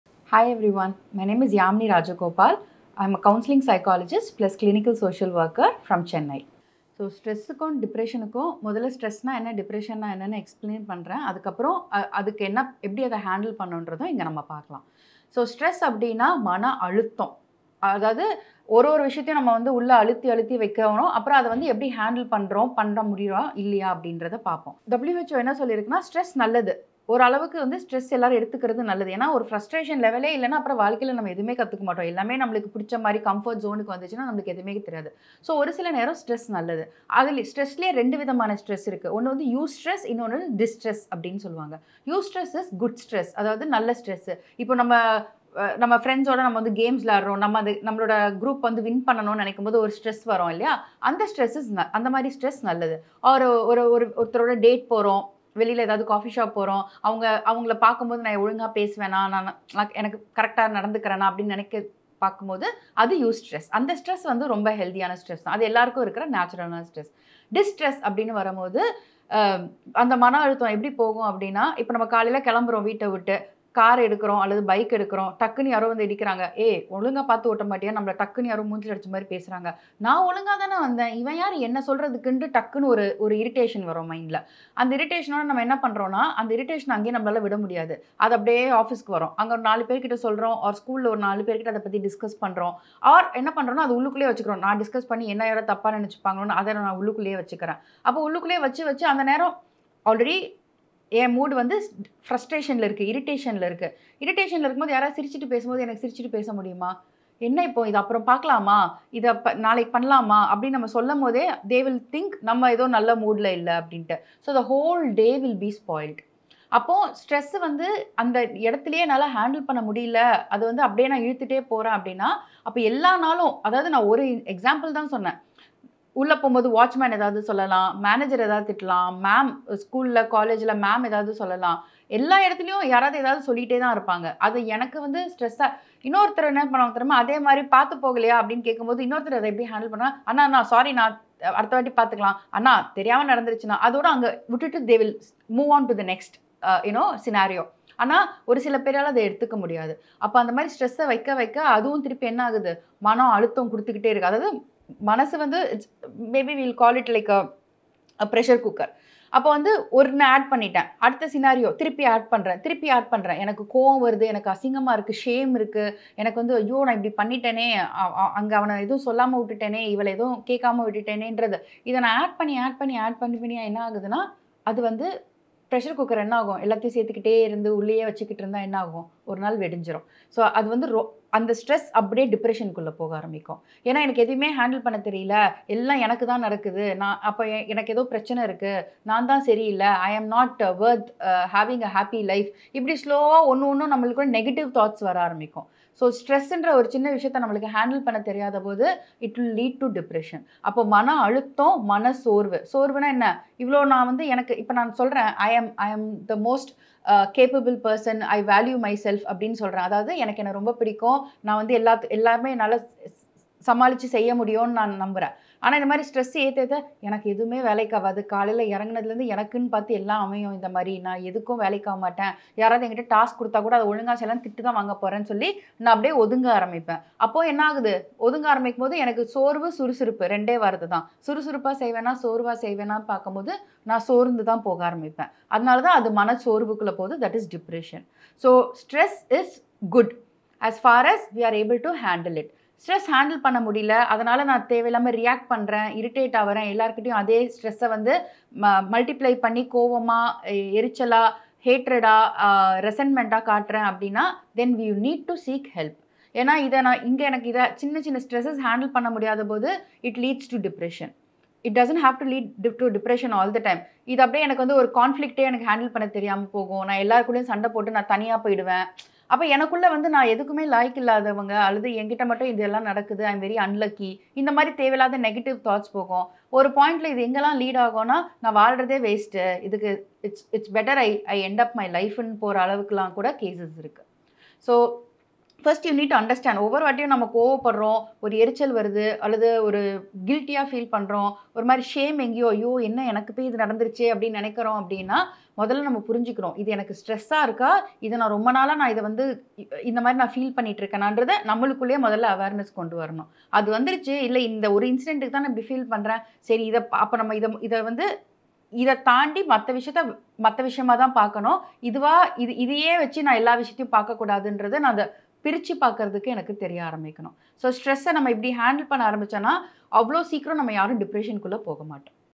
Stress-க்கும் Depression-க்கும் இது தான் வித்தியாசம் ! - Ms. Yamini Rajagopal
Address:
Cadence Hospital
No.291, Arcot road, Kodambakkam
Chennai - 600024.